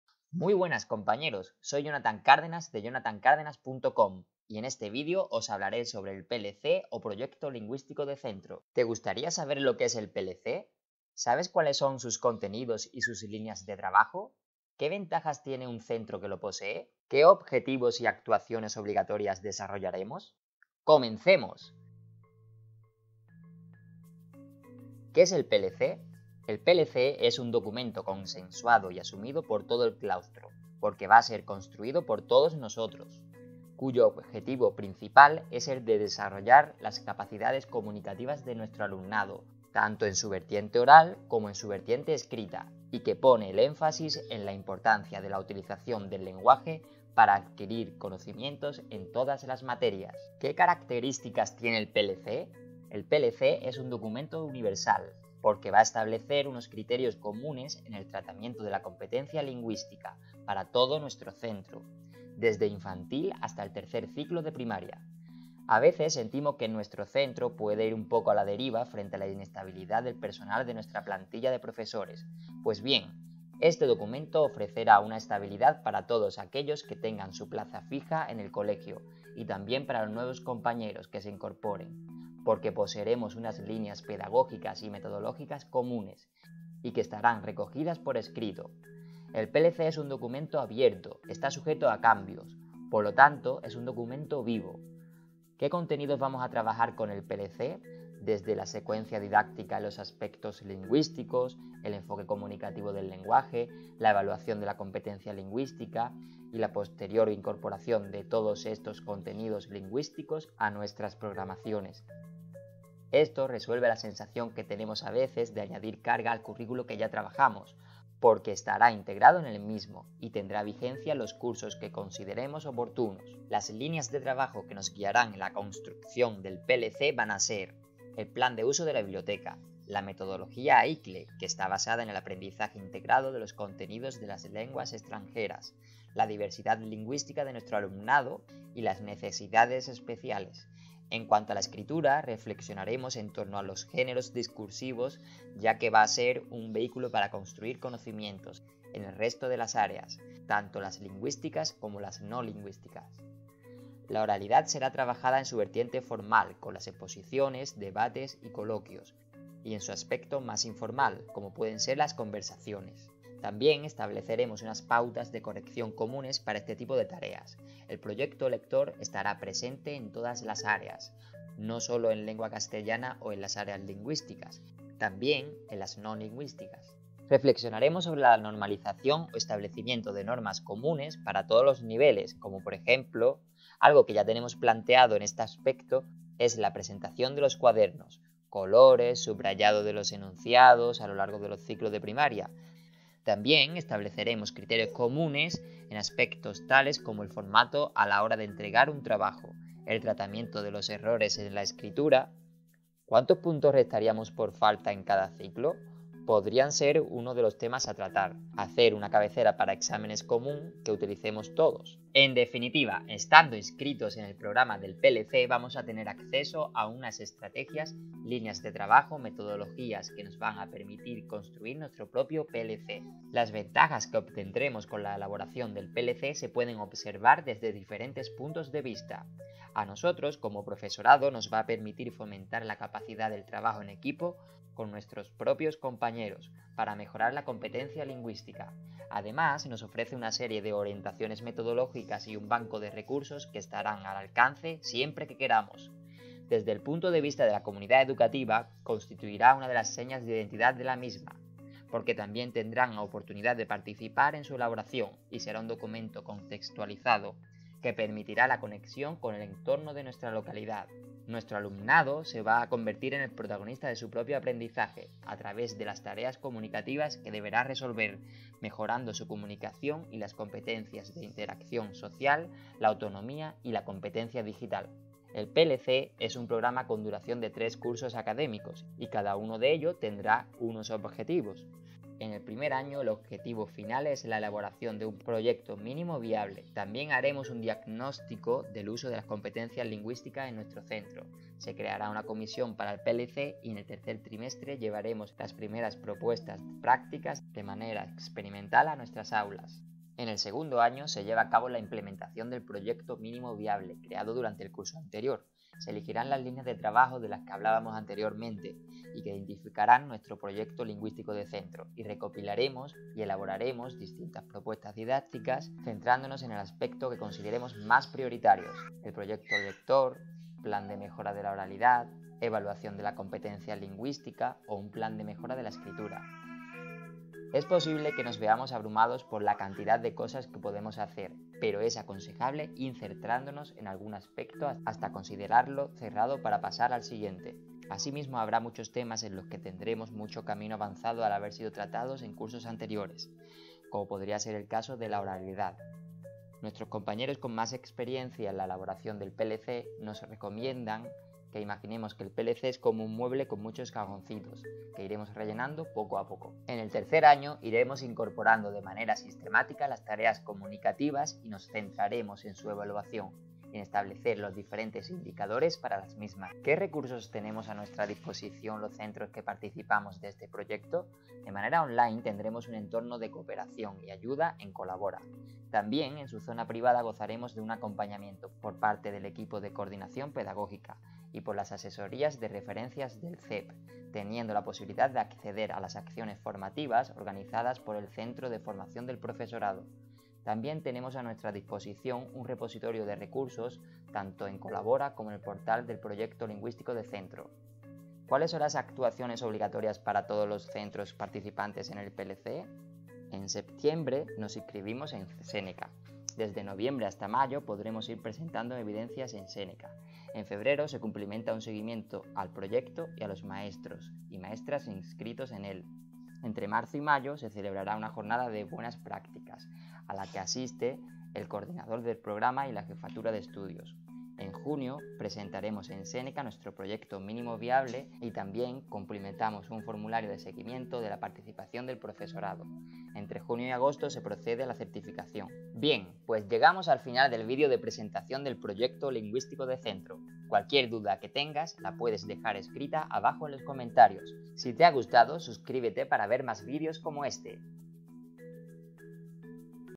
Introducción al Proyecto Lingüístico de Centro.
Vídeo que presenta el Proyecto Lingüístico de Centro a maestros y maestras de un centro de infantil y primaria que acoge el Proyecto en su primer año. Con este contenido se explica qué es el PLC, líneas de trabajo que seguiremos, contenidos de referencia, metodología, los objetivos en cada uno de los tres cursos  escolares en los que se desarrollan y las actuaciones obligatorias a lo largo del curso escolar.